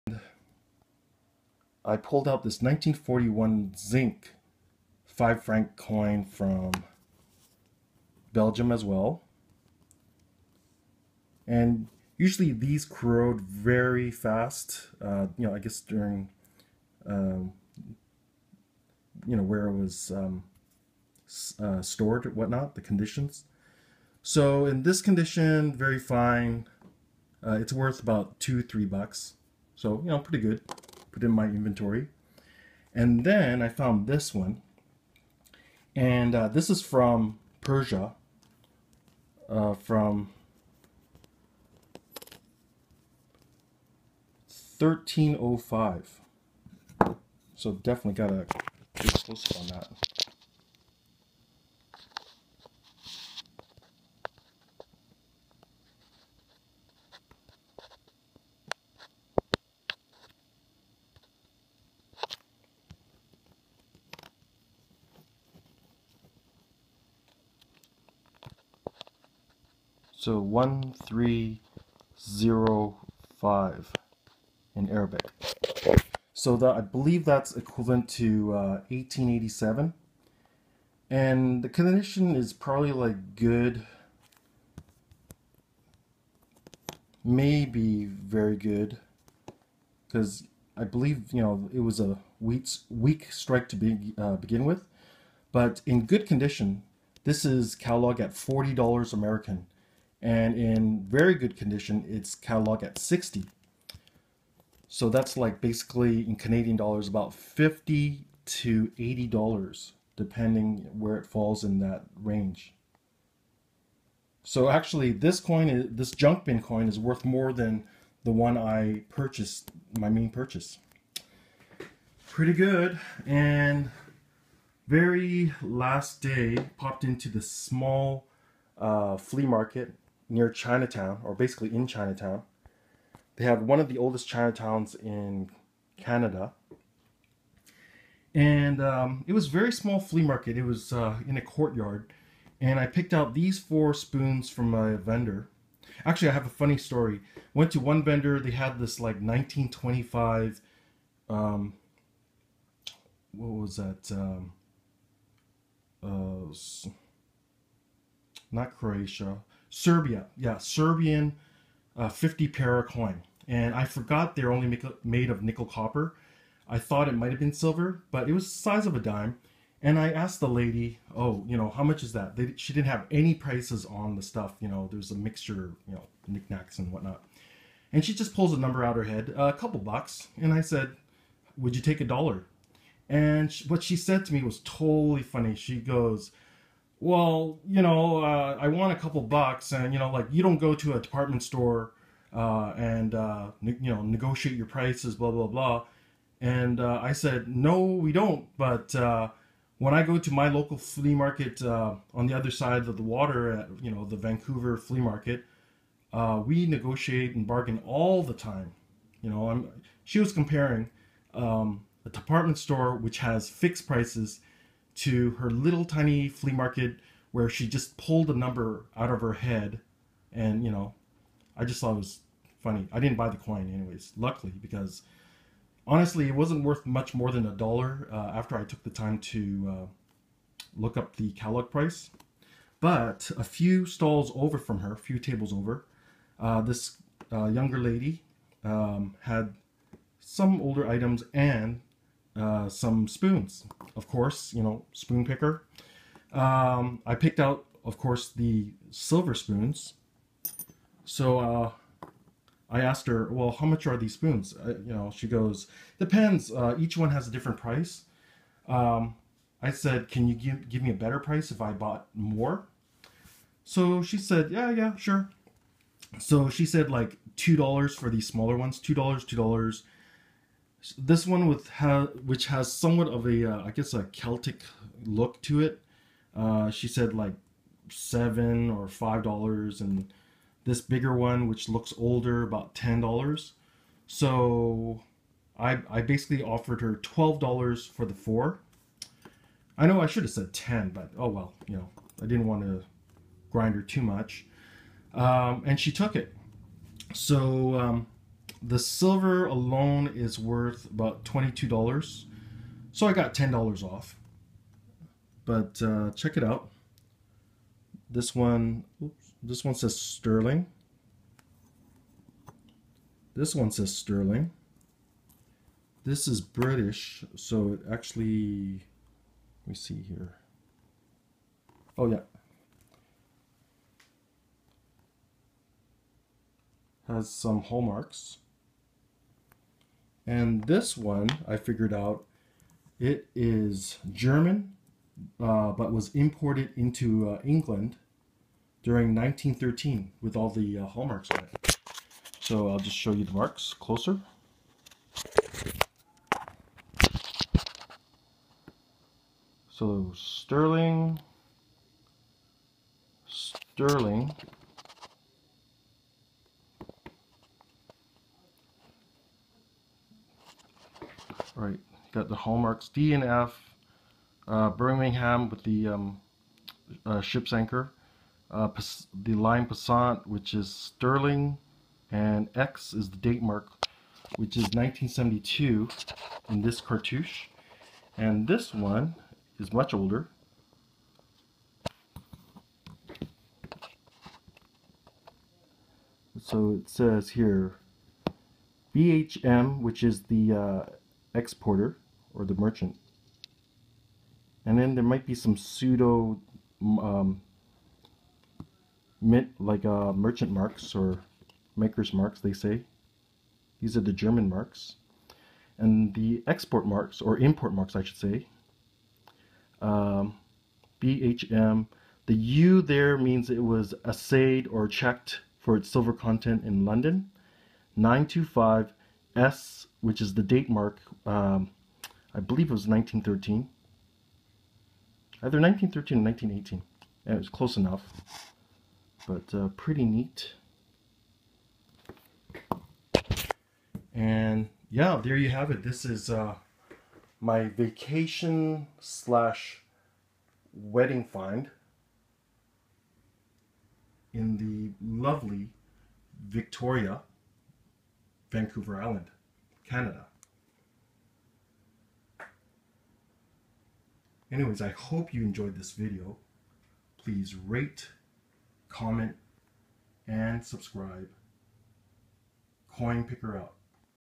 Coinpicker's Victoria, BC Vacation Pick Ups (Part 2 of 2)
Hi YouTube!  Here's the second half of my vacation finds from Victoria, BC.

Send to:

Coinpicker
c/o  S. Leung
Burnaby, BC V5C 6J4
Canada